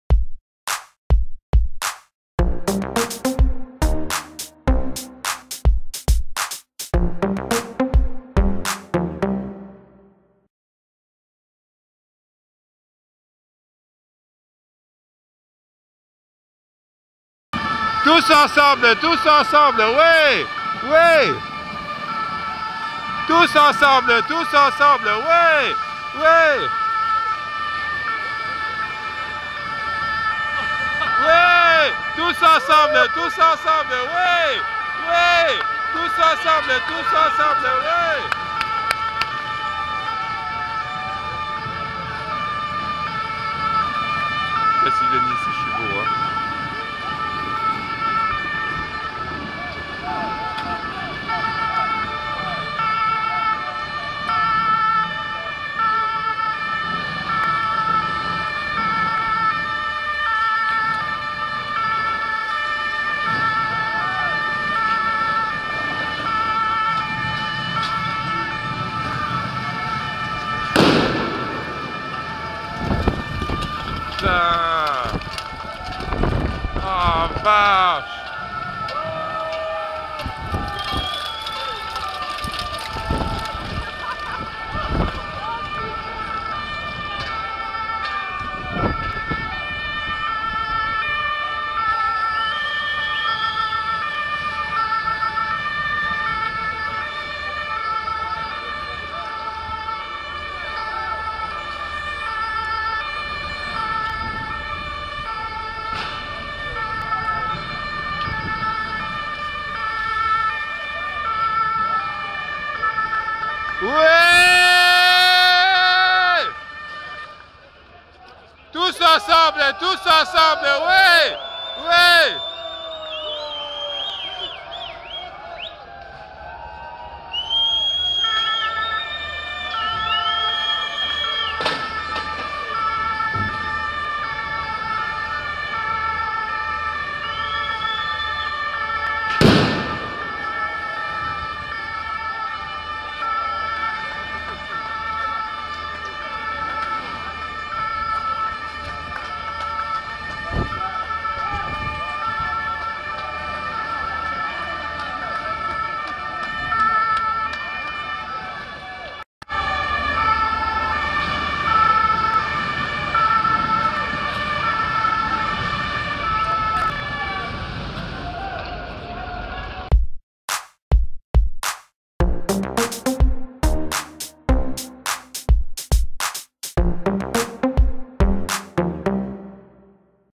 film manif bruxelles pour une europe sociale
Manifestation     4 avril 2014   à Bruxelles
avec tous les syndicats européens , contre l'austérité et pour une Europe Sociale